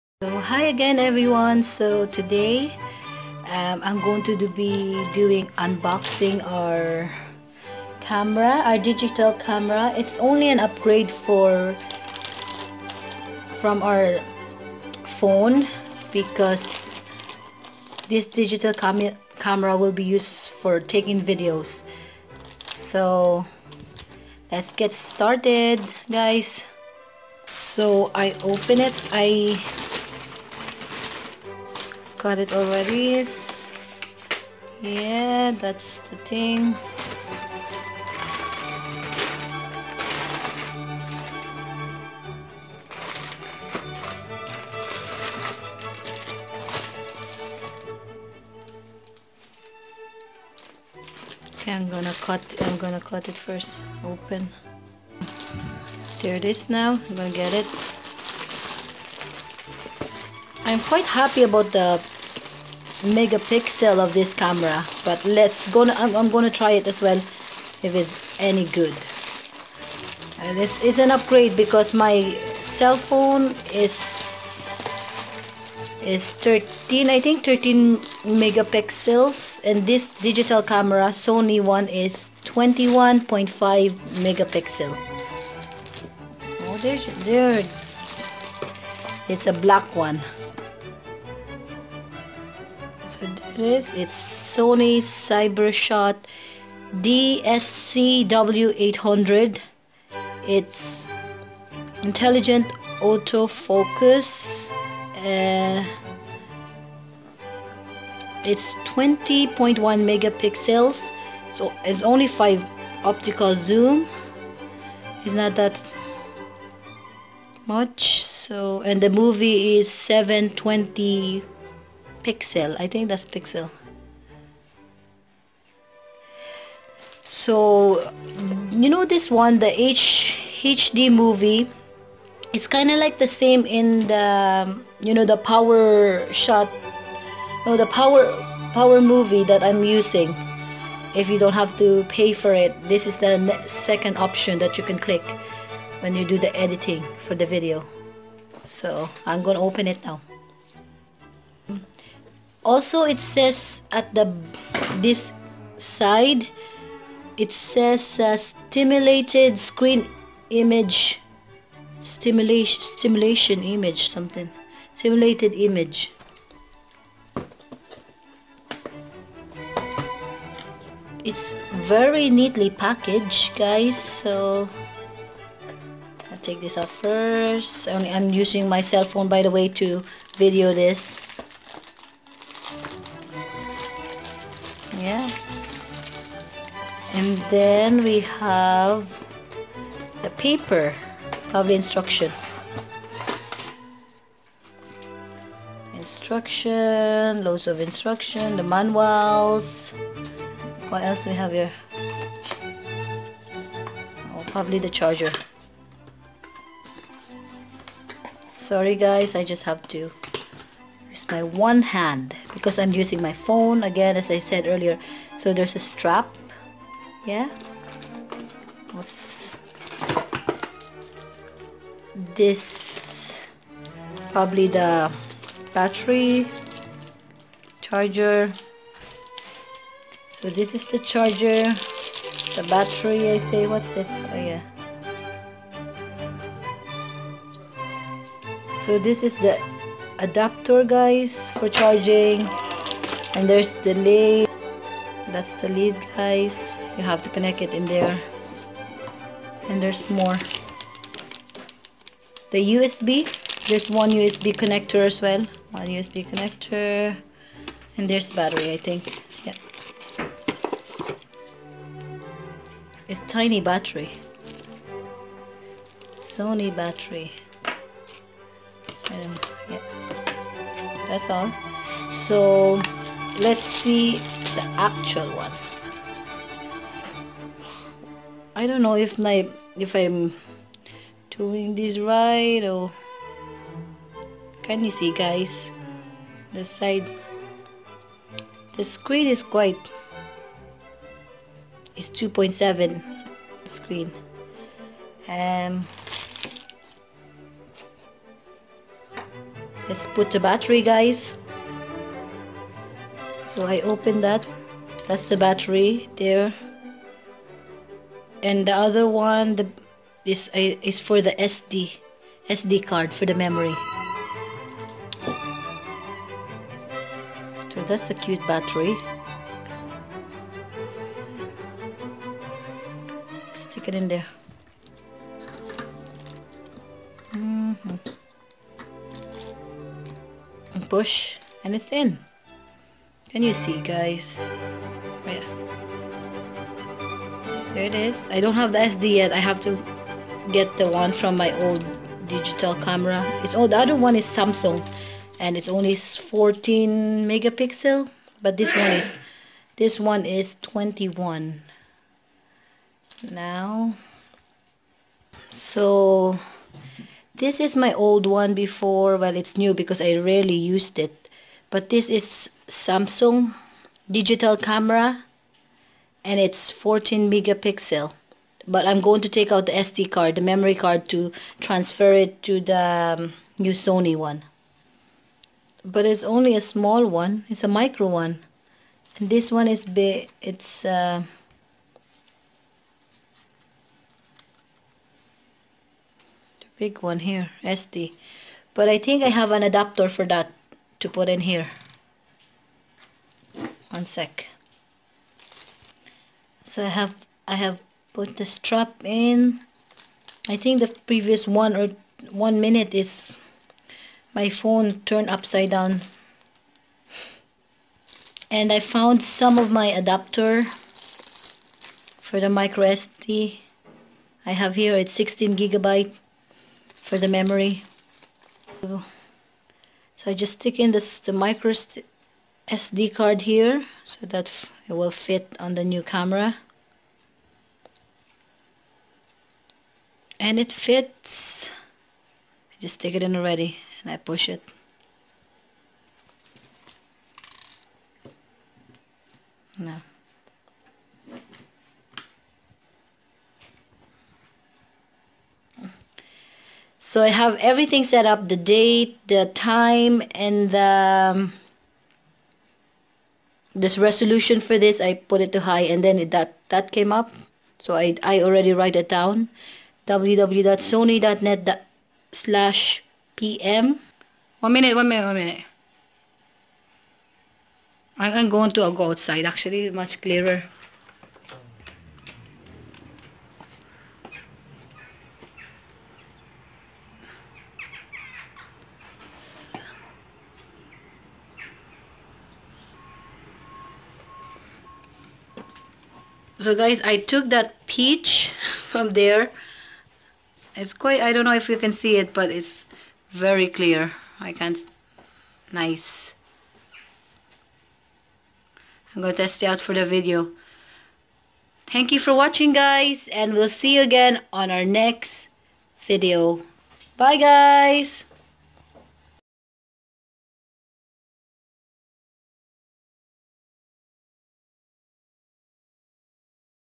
unboxing our Sony 20.1 megapixel camera | sonydsc-w800
Sony Digicam upgrade from our cell phone when we have to captured that videos for our family channel. With Much Love, Ciara Doolan & Family. 🇮🇪 ❤ 🇵🇭